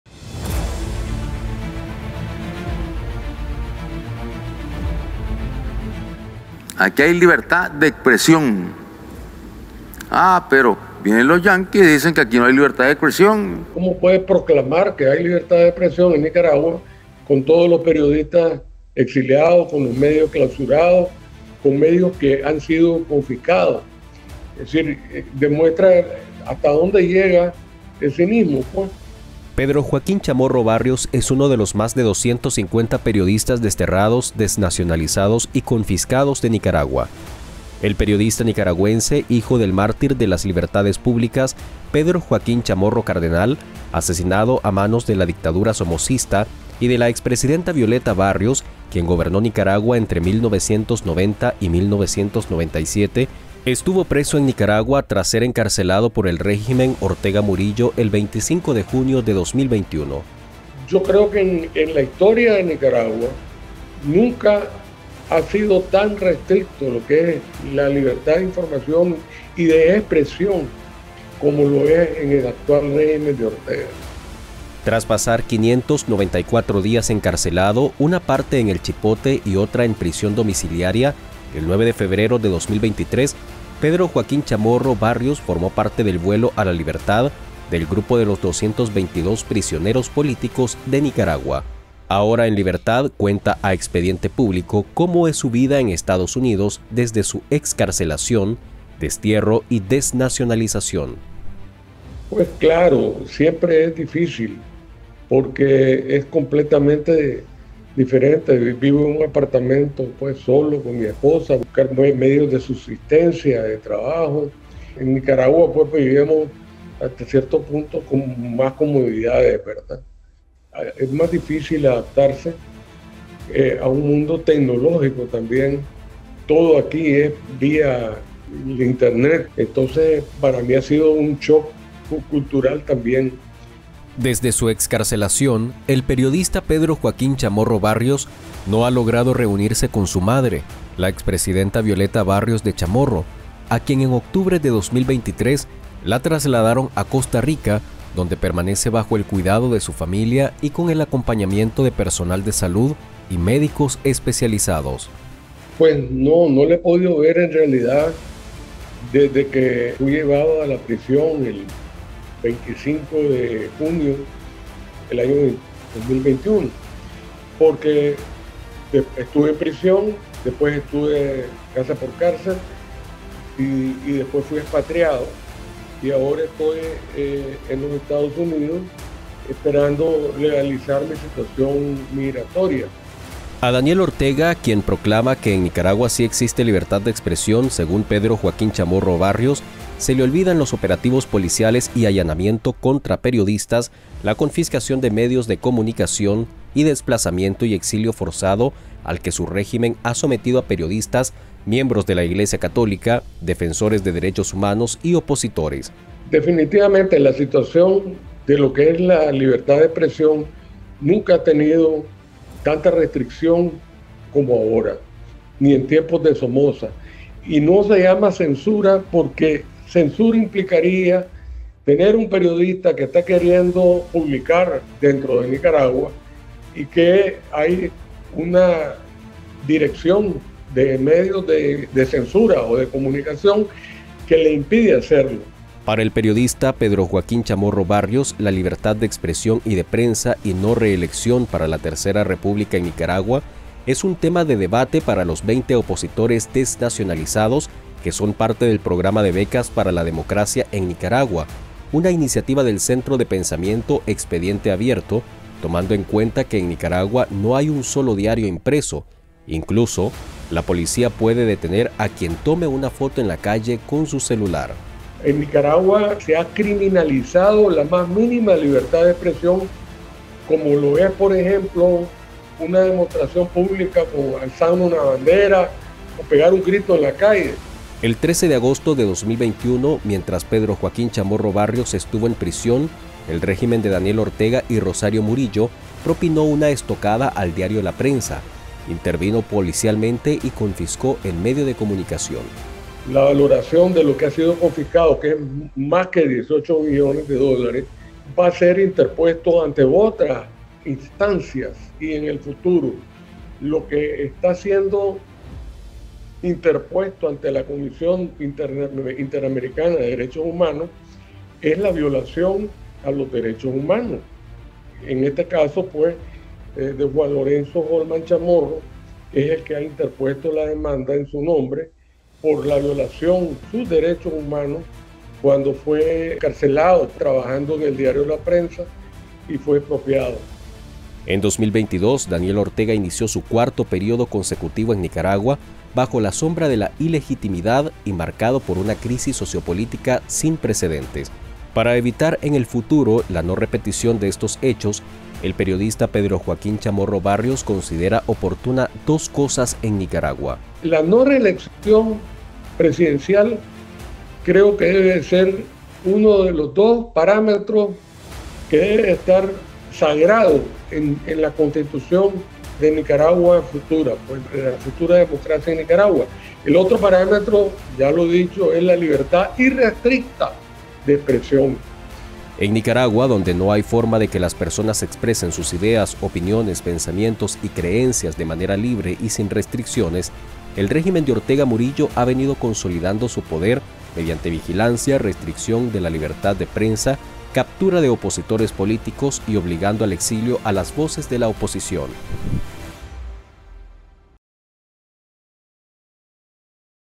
Pedro Joaquín Chamorro: “Nunca ha habido tanta restricción como ahora, ni en tiempos de Somoza”
¡Síguenos!